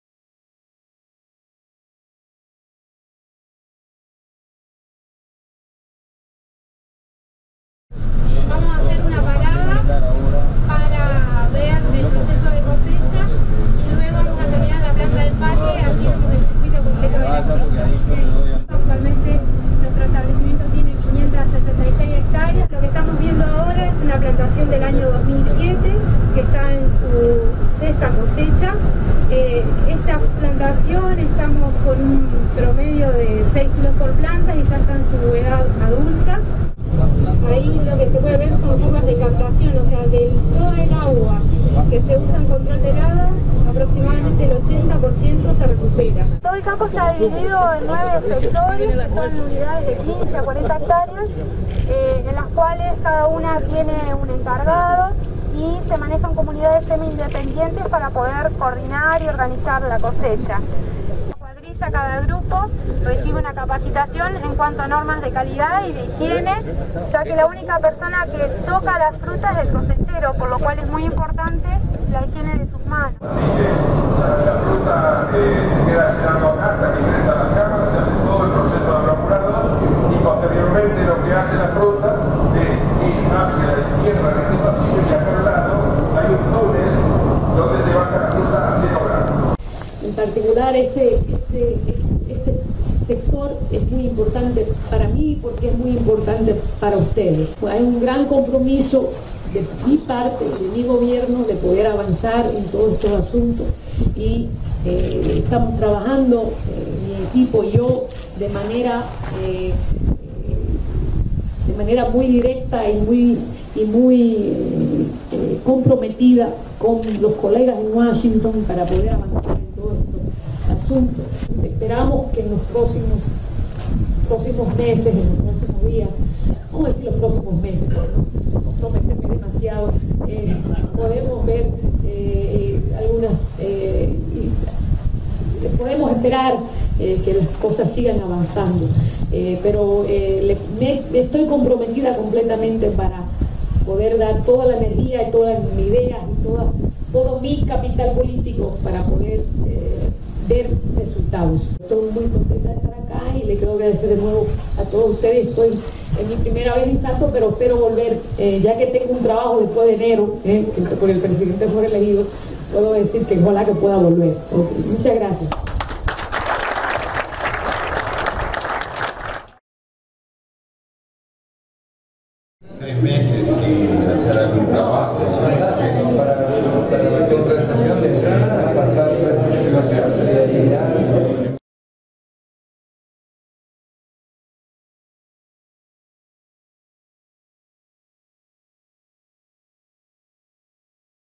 Visita de la embajadora Reynoso a plantaciones de arándanos en Salto
8 de noviembre de 2012.